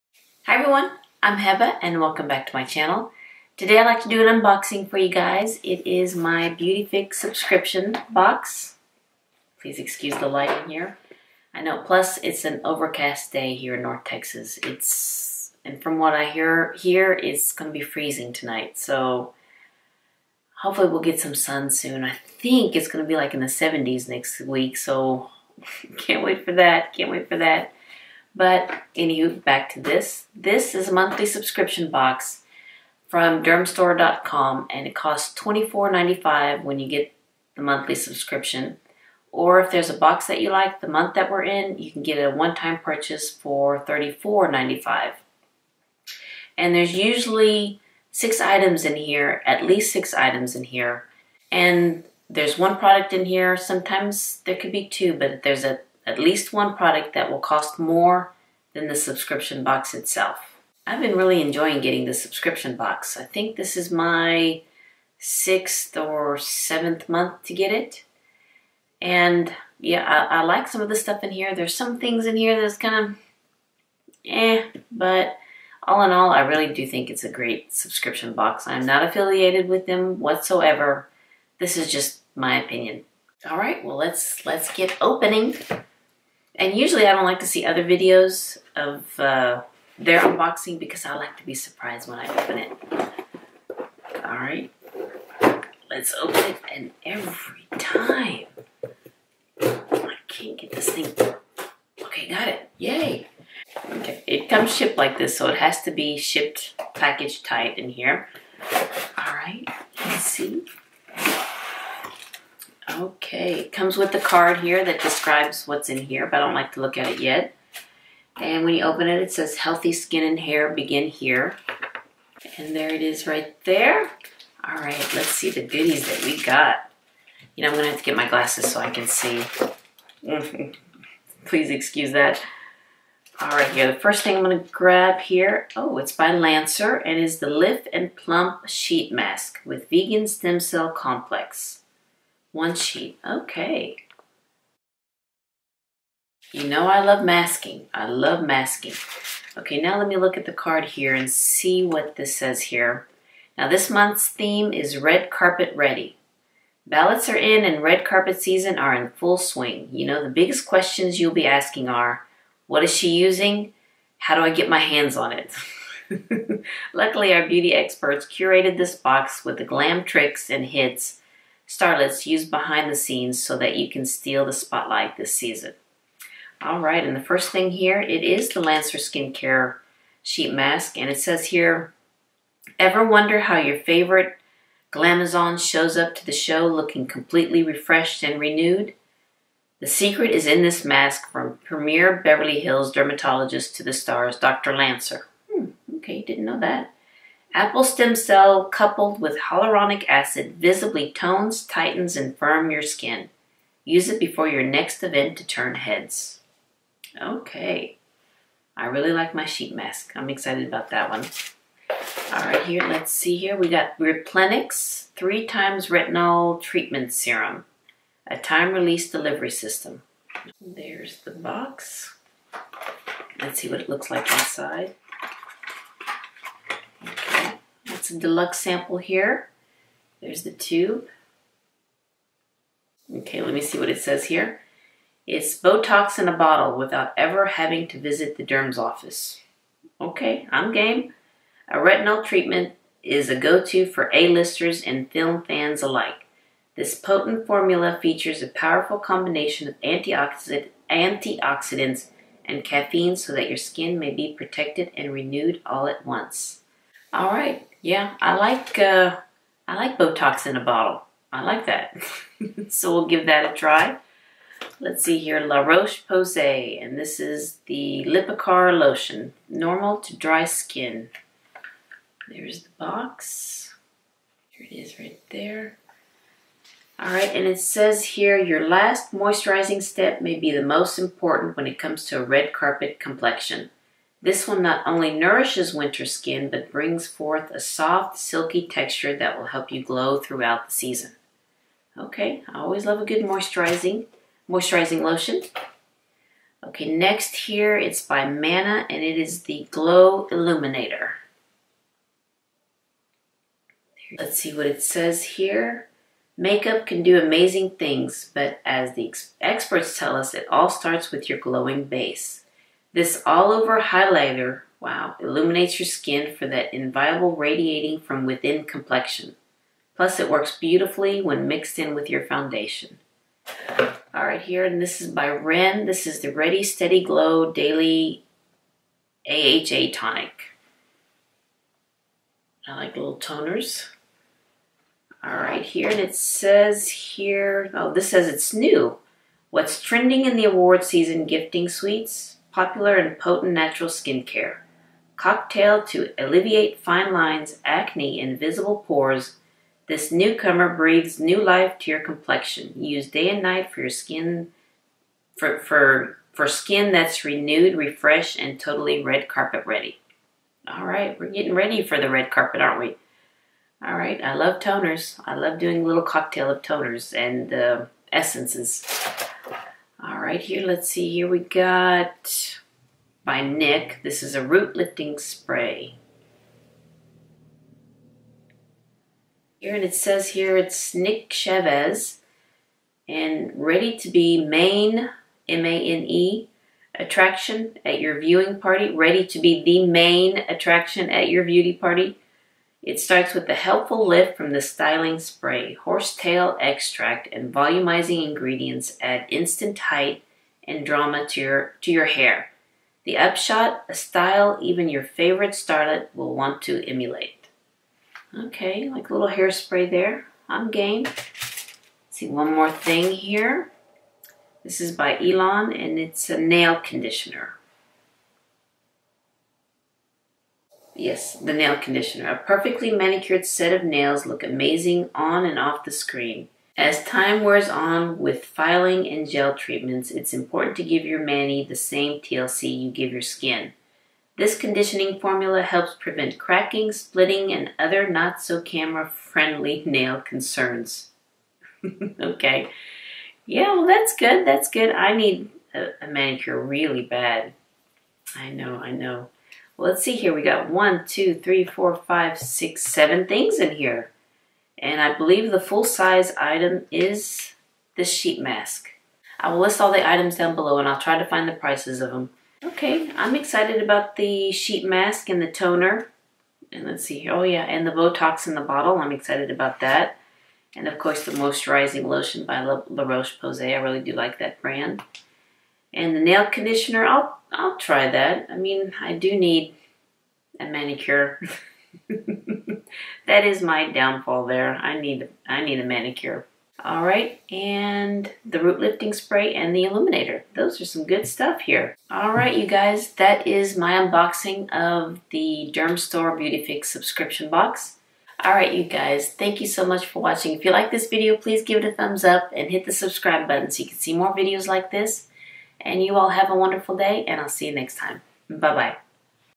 Beautyfix February 2018
Hi everyone!  Here is the Beautyfix unboxing for February 2018.  Enjoy!

These were bought by me with my own money and are my own views.